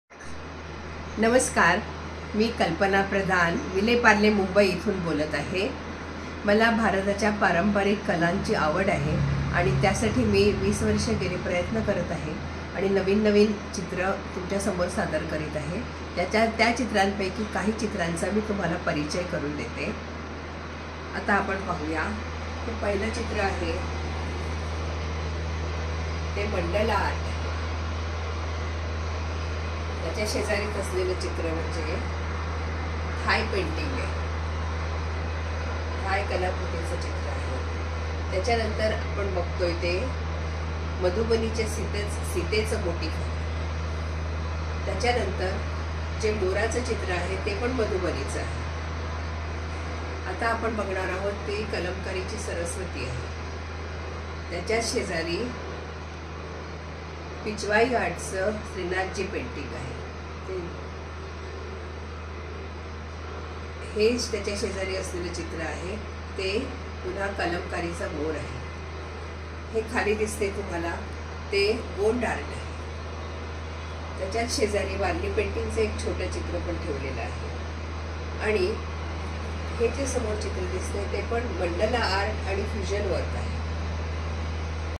Kalpana Pradhan Mumbai
Kalakaushalya Celebrating International Women's Day : On the occasion of International Women's Day, we are organizing a special event showcasing artistic talents. This event aims to provide an opportunity for female members of the artistic skills group to share information about their art/craft or business through videos.